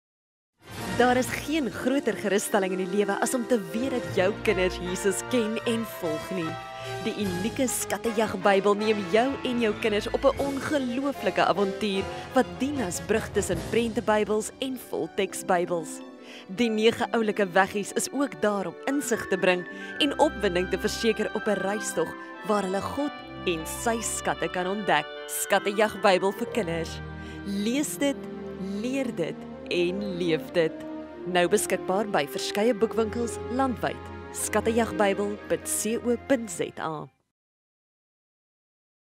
SKATTEJAGBYBEL - ADVERT
Daar is geen groter gerusstelling in die lewe as om te weet dat jou kinders Jesus ken en volg nie. Baie mense sukkel om die Bybel te verstaan of om God in Jesus persoonlik te leer ken en party gee soms al vroeg in hul lewe op...
Wonder jy nie hoe kosbaar dit sou gewees het, as jy al as kind saam met iemand die skatte van die Bybel kon ontdek, of God kon leer ken het in lyn met wie jy is en hoe jy dink?
Die unieke Skattejagbybel neem jou en jou kinders op n ongelooflike avontuur wat dien as brug tussen Prentebybels en Volteksbybels. Die 9 oulike “Weggies” is ook daar om insig te bring en opwinding te verseker op n reistog waar hulle God en Sy skatte kan ontdek.
Skattejagbybel - Lees dit, leer dit en leef dit.